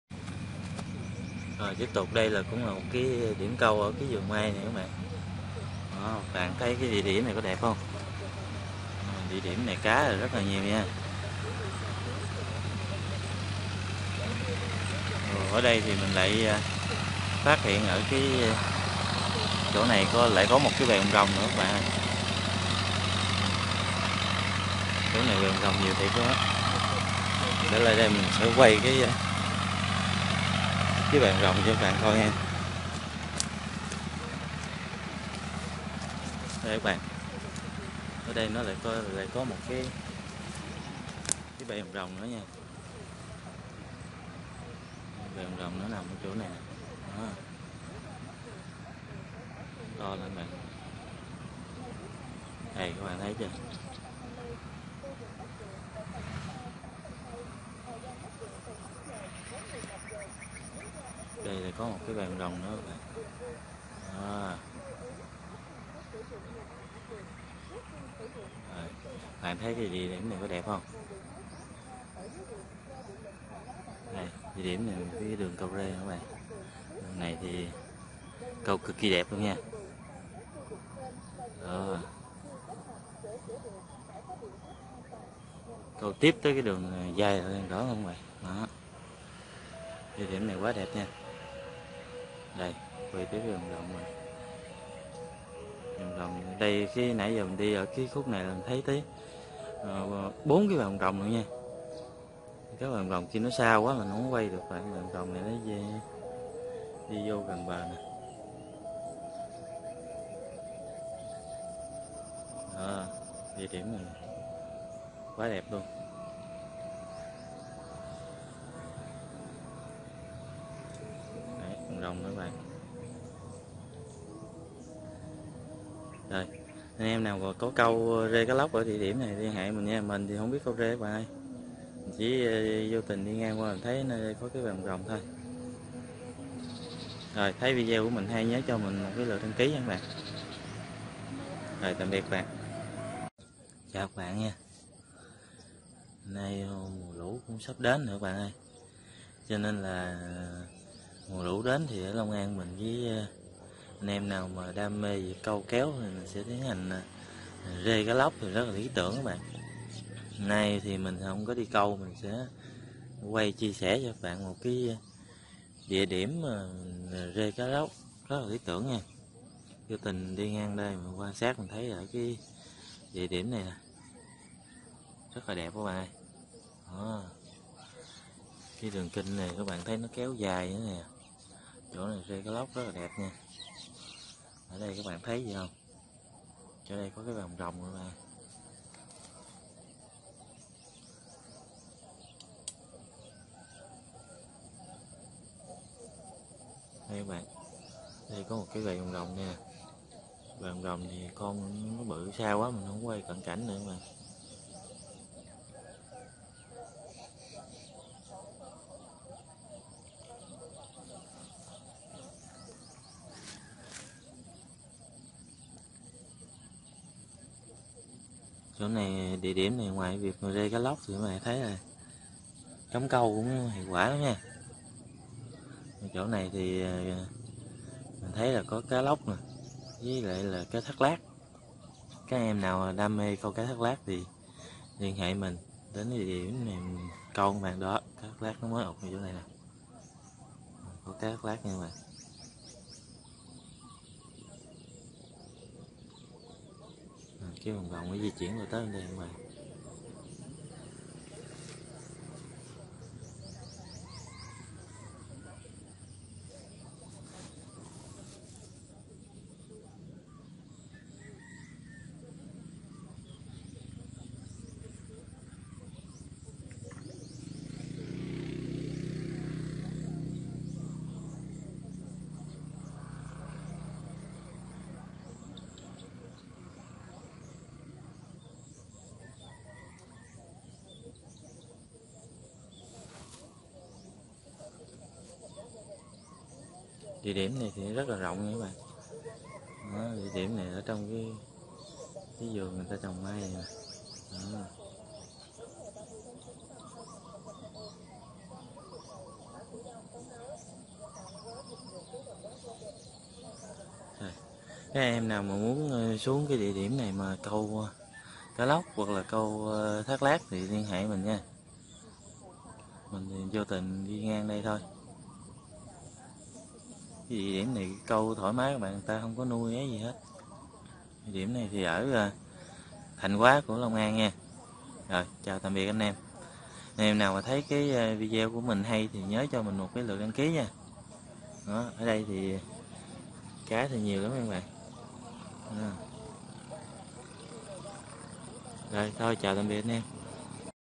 LŨ VỀ RỒI, RÊ CÁ LÓC THÔI ANH EM, kênh săn bắt miền quê VP, TẬP 14 cHIA SẼ ĐIỂM RÊ CÁC LÓC RUỘNG MAI
Re ca loc.